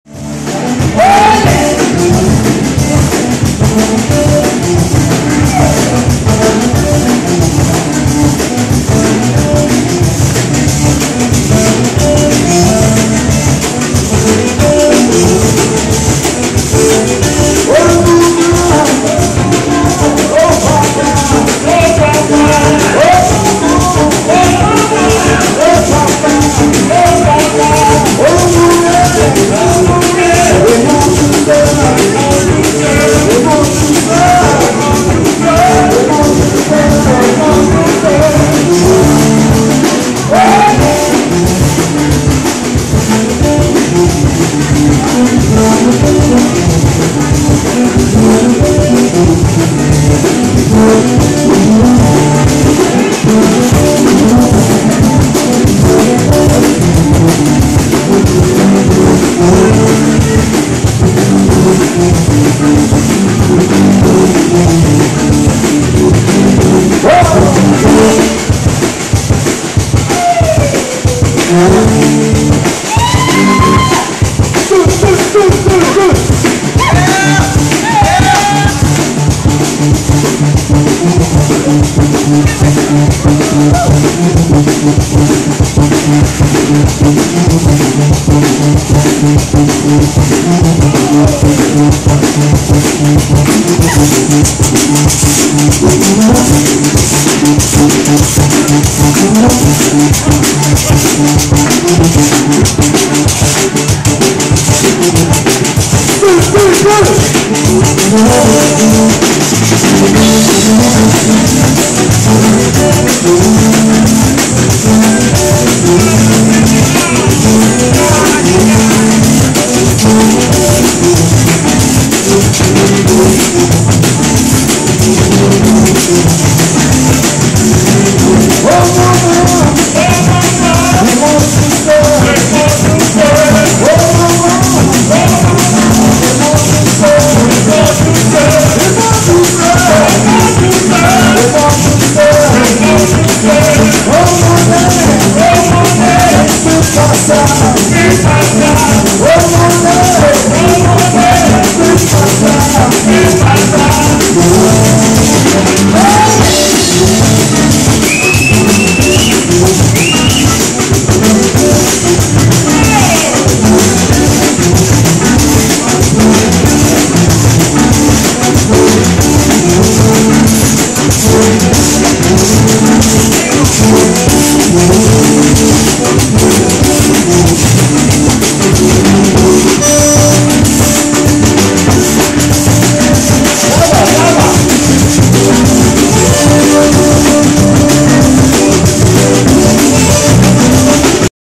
Roy Carrier - Zydeco
Roy Carrier  - Zydeco musician from Lawtel, LA playing at the German Club in RI for Sal's Productions